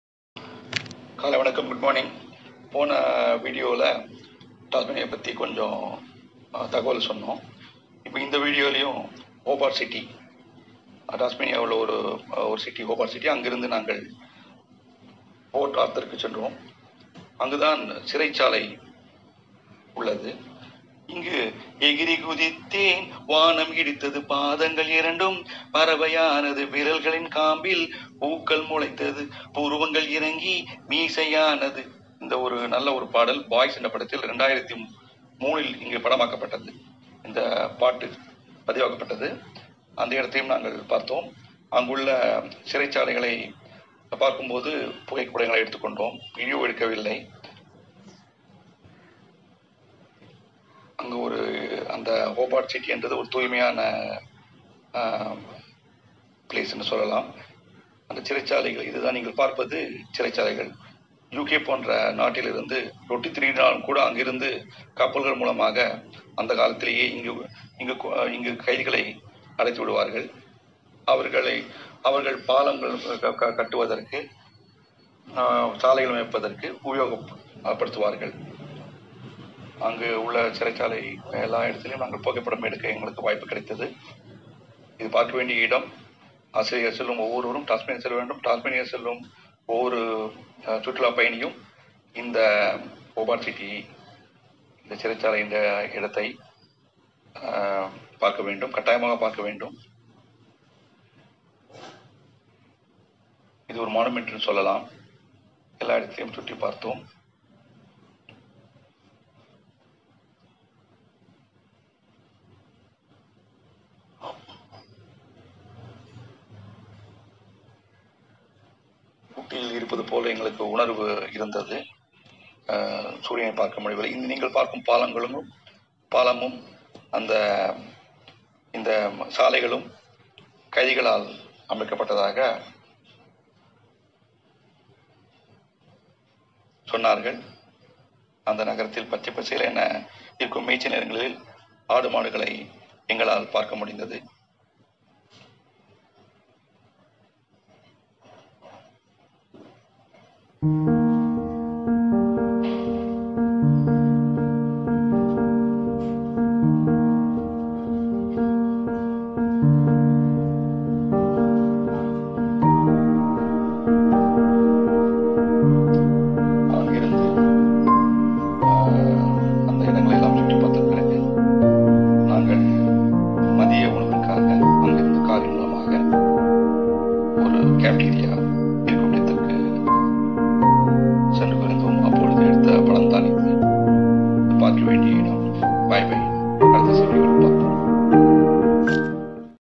Tasmania Tour - Part -2
Visit to Tasmania, Australia- Port Arthur, Town of Tasmania.